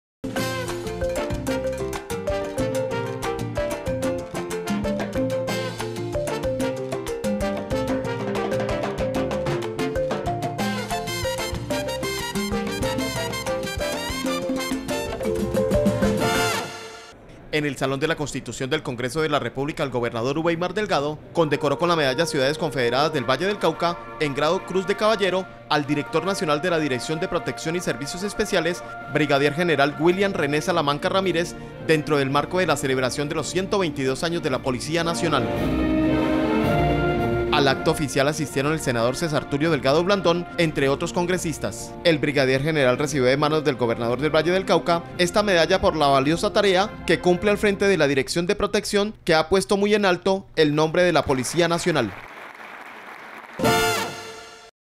Ubeimar condecoró en los 122 años de la Policía Nacional al Director Nacional de Protección
En el salón de la Constitución del Congreso de la República, el Gobernador del Valle del Cauca, Ubeimar Delgado Blandón condecoró con la medalla "Ciudades Confederadas del Valle del Cauca, en grado Cruz de Caballero", al Director Nacional de la Dirección de Protección y Servicios Especiales, Brigadier General William René Salamanca Ramírez, dentro del marco de la celebración de los 122 años de la Policía Nacional.

Dicha distinción se otorga a las personas y entidades que han prestado eminentes servicios al departamento del Valle del Cauca. Al acto oficial asistieron el Senador César Tulio Delgado Blandón y el representante  Henry Arcila, entre otros Congresistas.

El Brigadier General recibió de manos del Gobernador del valle del Cauca esta medalla  por la valiosa tarea que cumple al frente de la Dirección de Protección que ha puesto muy en alto  el buen nombre de la Policía Nacional